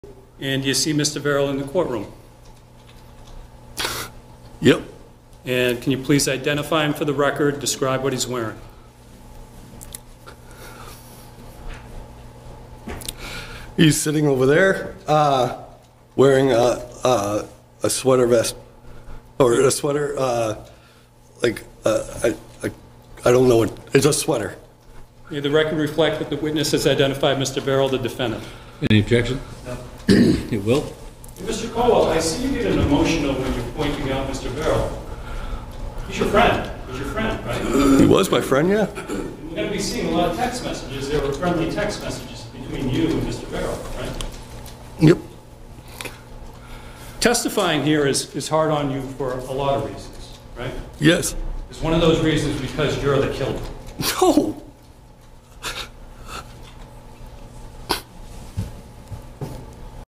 [PT 2] Witness Breaks Down on the Stand | NH v.
"Did you slash their throats?" Prosecutor Peter Hinckley triggers an emotional reaction from witness Joshua Colwell during his testimony in the SmallTownSecretsMurderTrial. Colwell is the former drug associate of victim Christine Sullivan.